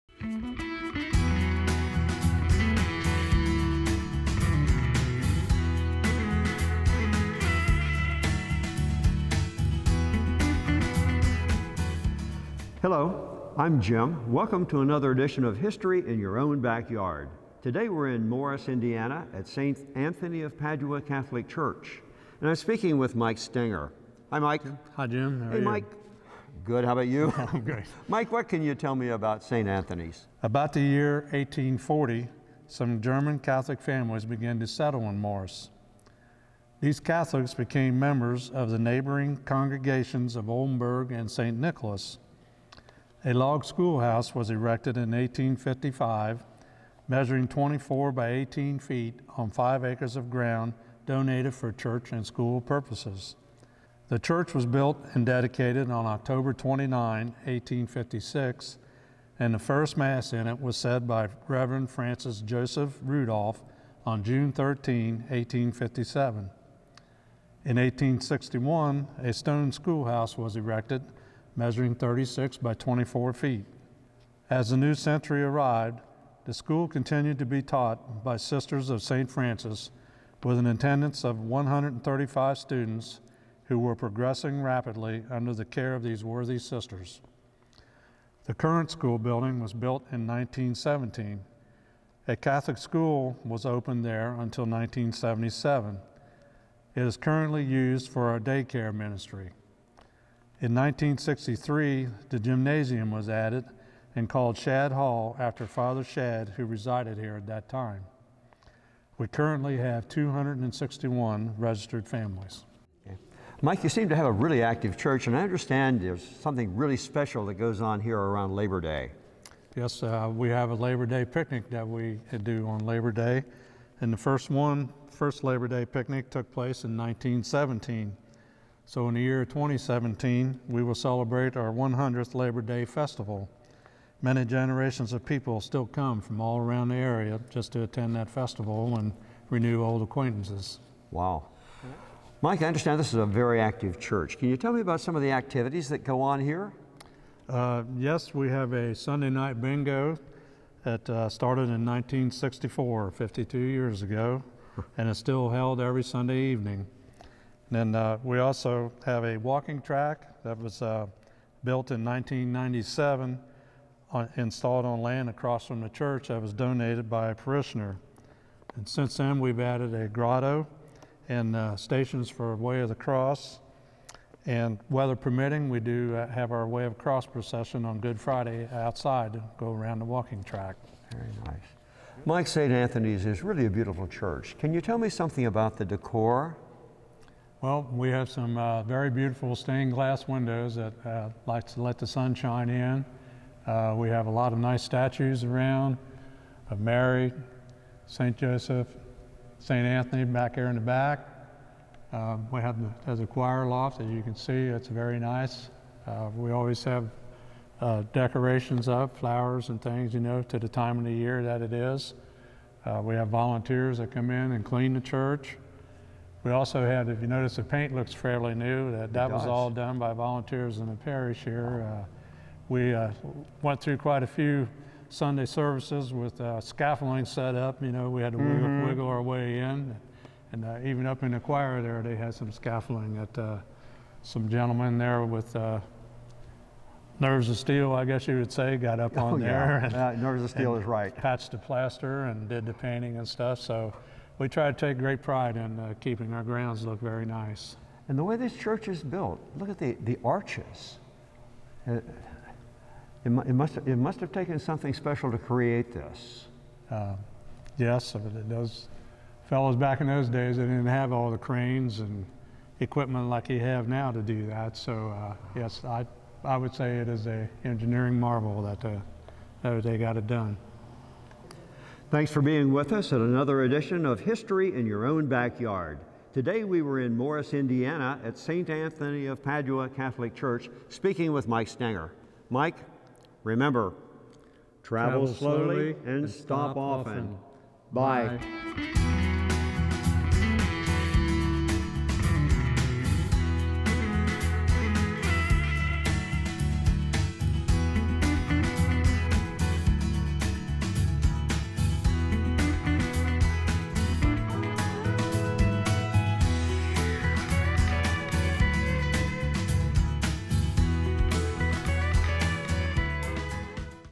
St Anthony of Padua Catholic Church
Many German Catholic immigrants left their homeland to escape the turmoil caused by Napoleon.  The Cincinnati area attracted a lot of Germans in the 1820's 30's. Some of these people had been farmers in Germany, so they were seeking rural areas to begin a new life in their new country.  At that time, Indiana farmland around here could be had for $350 for 80 acres.  The new settlement of Springfield looked very promising to them, except that the nearest Catholic Church was a five mile walk to Oldenburg.

In 1834, Pope Gregory XVI established the Diocese of Vincennes, with Father Simon Brute as the first bishop.  This occassionally brought traveling priests, sometimes called circuit riders, to the area to celebrate Mass and administer the sacraments.  Bishop Maurice de St. Palais purchased five acres of land from John and Susannah Miller in 1853.  This became the location of St. Anthony's Church.  That same year the first train arrived in Springfield.

In 1854, several prominent families met to determine how best to build St. Anthony Church.  Many of today's parishioners are descendants of those early families.  In 1855, a log school house was built on church property.  Some people beleived that a Catholic school was so important that is should be built even before the church building was finished.  On October 29, 1856, Bishop de St. Palais blessed the first St. Anthony Church.  In 1861, a new stone schoolhouse was built and a frame house was put up for the teaching sisters.  In 1866, Father Roman Weinzaepfle became our first resident pastor.  In 1868, the parish installed two new bells for the bell tower, the larger dedicated to Mary Immaculate and the smaller to St. Joseph.  An organ was purchased in 1882.  When the village was officially platted in 1858 the name was changed to Morris.

In 1963 Schad Hall, after Father Albert Schad, was built.  In 1964, Mass began to be said in English instead of Latin.  In 1965, the teaching sisters got a new convent, but St. Anthony's grade school later closed in 1977.

Father(later Monsignor) Bernard Schmitz came to St. Anthony's in 1970.  Father was a very spiritual pastor who provided Holy Hours, Eucharistic adoration and many other opportunities for us to grow in our faith and our love of God.  Father loved his German heritage and encouraged the writing of our parish history.  Father was innovative and very hard working.  He believed in keeping the parish facilities in good repair.

After Msgr. Schmitz retired in 2006, Fr. Pascal Nduka took the helm and continued our good work until November, 2013.  Fr. Pascal is from Nigeria and he helped us learn to be more globally aware.
Fr. Shaun Whittington served as pastor from 2013 to 2020.  Father continued with the programs, organizations and activities for which St. Anthony is well known.

Video  Release  Date:  November  2,  2016